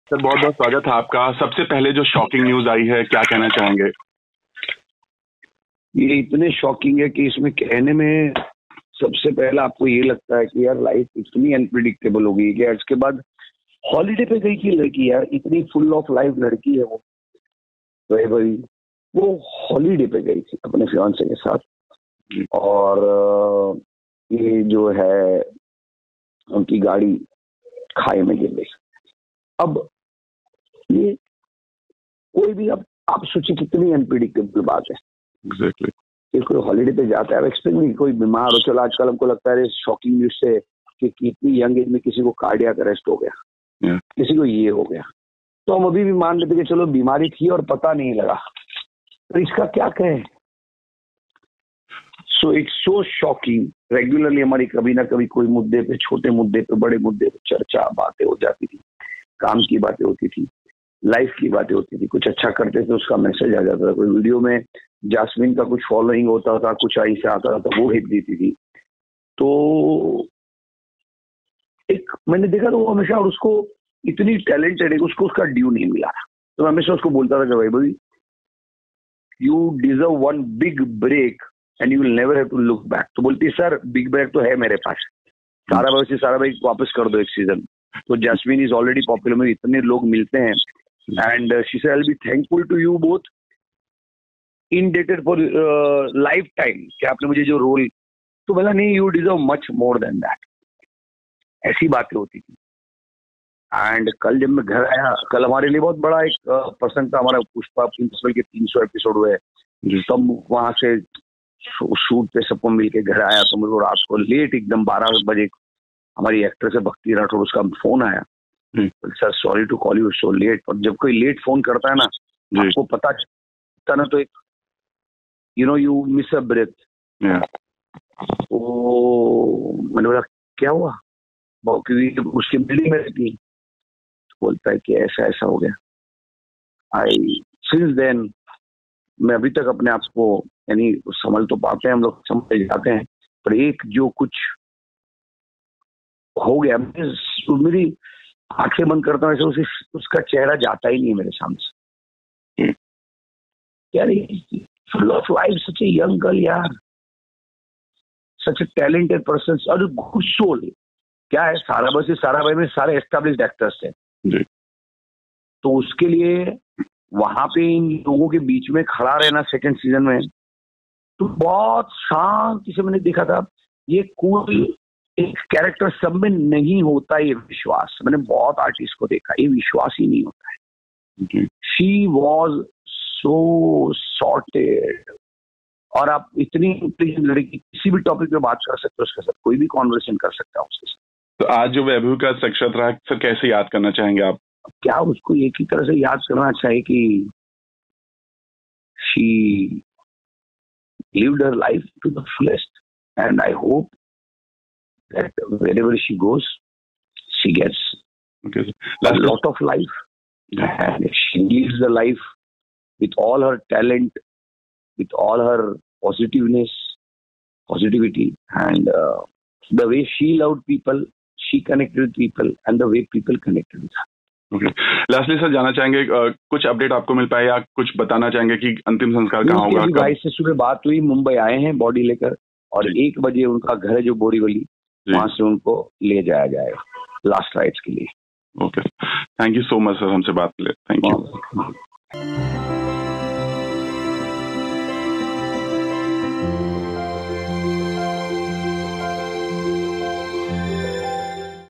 Sarabhai vs Sarabhai actor Vaibhavi Upadhyaya death: JD Majethia shares FULL details of accident
Telly News - Sarabhai vs Sarabhai fame actor Vaibhavi Upadhyaya died suddenly in a car accident. JD Majethia in an exclusive conversation with Telly Talk shares full details of the accident and expresses shock over her sudden death.